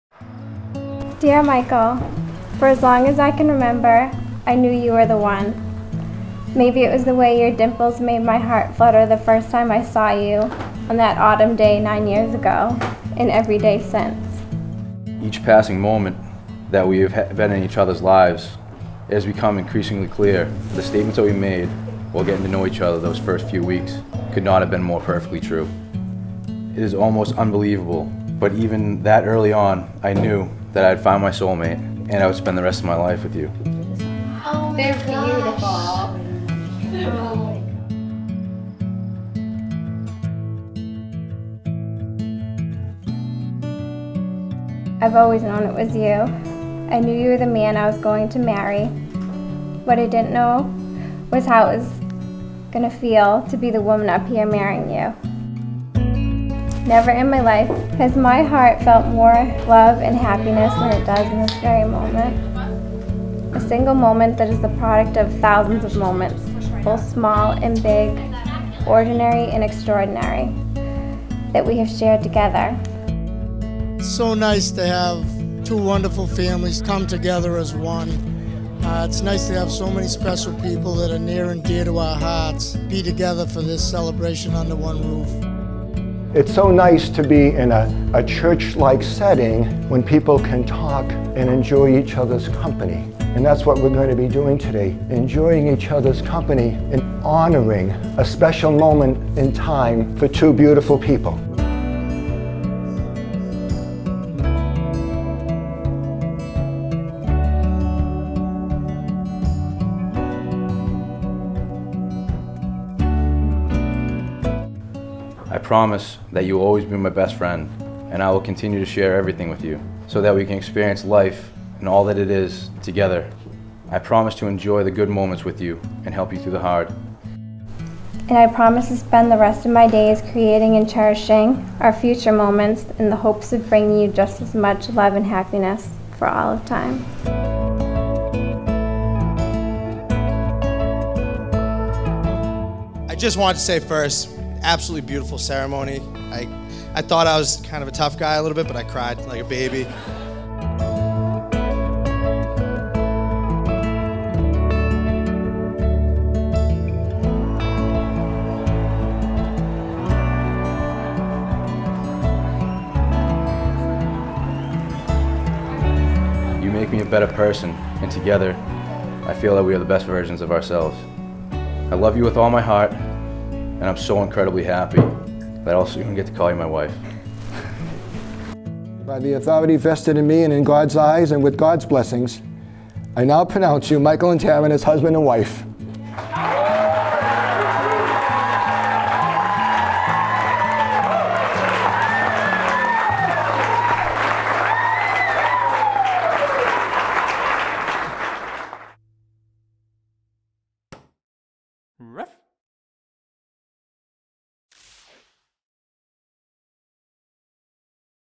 Taryn & Mike's emotional Same Day Film
Some of my favourite things about Taryn+ Mike's wedding aside from their amazing venue (Bishop Farm Weddings & Events) are -the weather, it was a crisp and beautiful autumn day - Taryn's dress... was absolutely stunning - The ceremony space, Bishop Farm converted their "cow shed" to a rustic indoor ceremony space complete with stained glass windows and beautiful pews - Taryn & Mikes vows, I may have cried a bit - and lastly, when a guest of the wedding came up to me after watching this Same Day Film and said "that was @!*# ing amazing I cant believe you did that so quickly"

The Team:

cinema: amanda, emma & tess
editor: amanda

catering: The Common Man
cake: White Mountain Cupcakery
dj: Sowa Entertainment
flowers: Cherry Blossom Floral Design
hair & makeup: Fresh Salon and Day Spa
photography: J. Harper Photography

|| copyright: readyset[film] ||